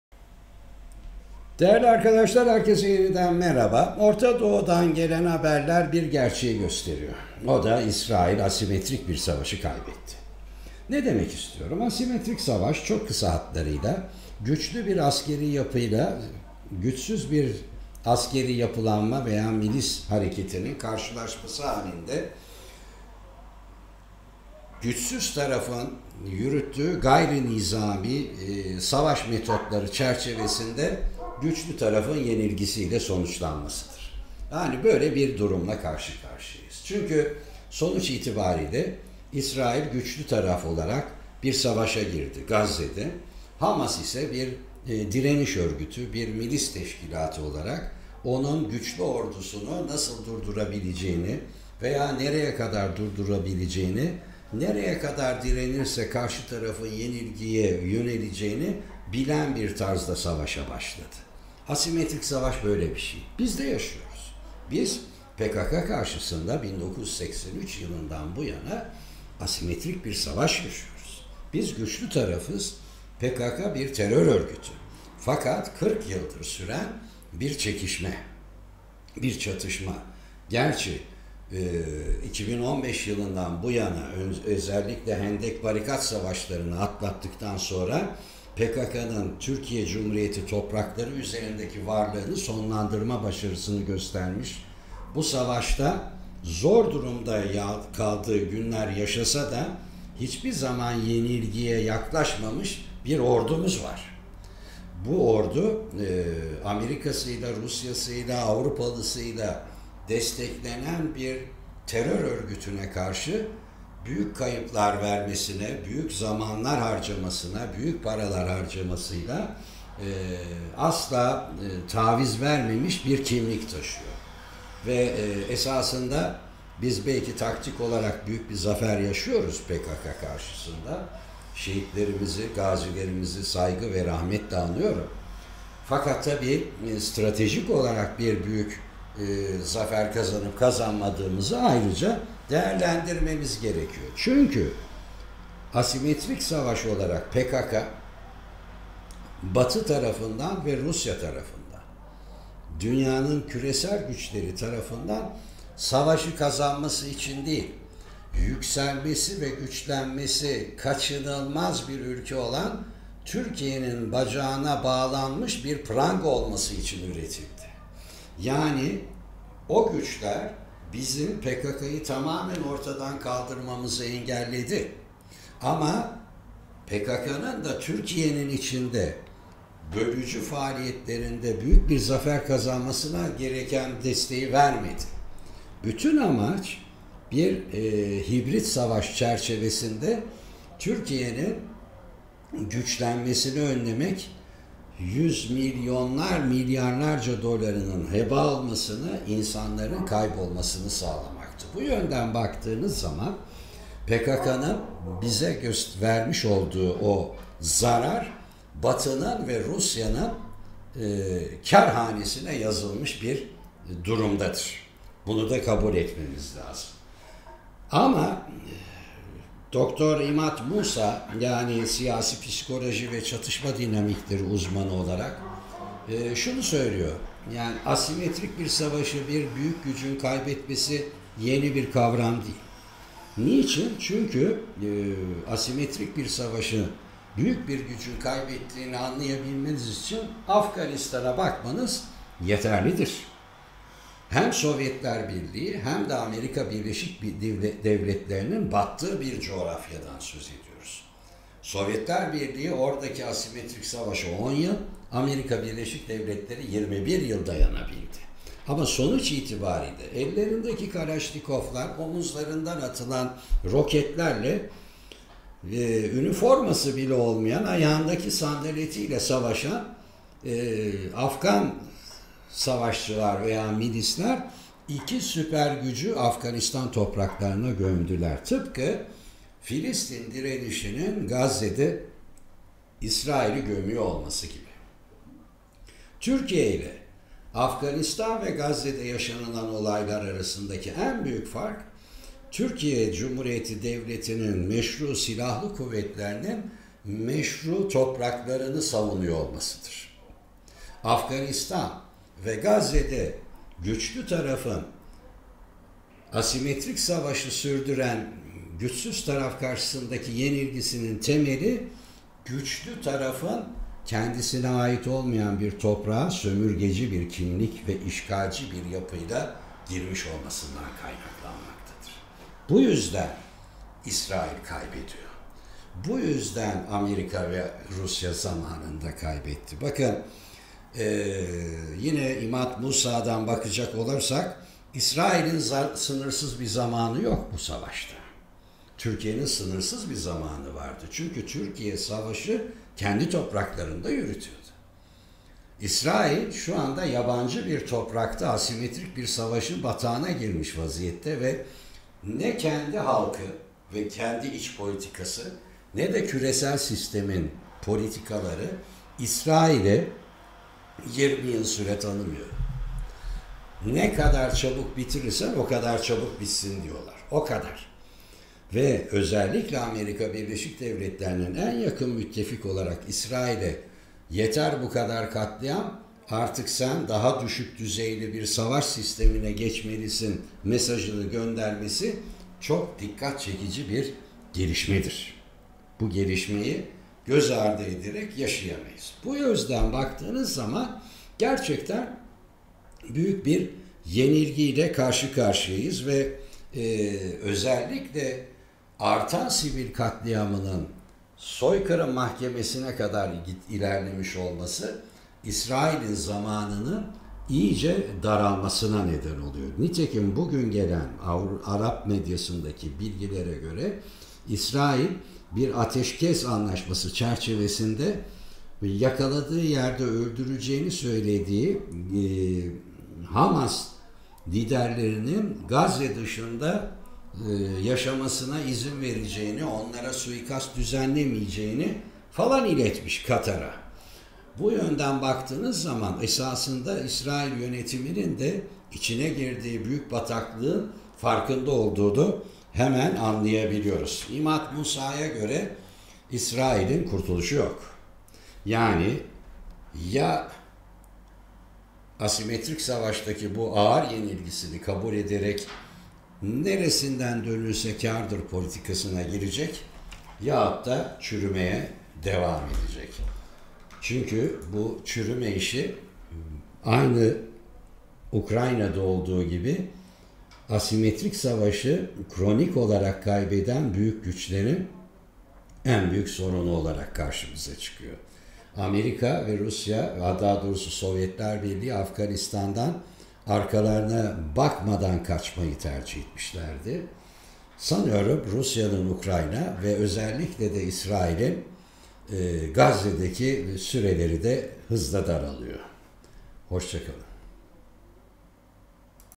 UZMAN TEŞHİSİ: "İSRAİL ASİMETRİK SAVAŞTA YENİLDİ, KURTULUŞU CEPHE GENİŞLETMEKTE ARIYOR!" ÖNEMLİ!..
İsrail yenilgisini hazırlayan ana etken ne?
İşgalci büyük güç, neden, işgal ettiği coğrafyada yenilgiye mahküm kimlik taşıyor?
İsrail'in neden zamanı daraldı, manevra alanını hangi unsurlar sınırlıyor?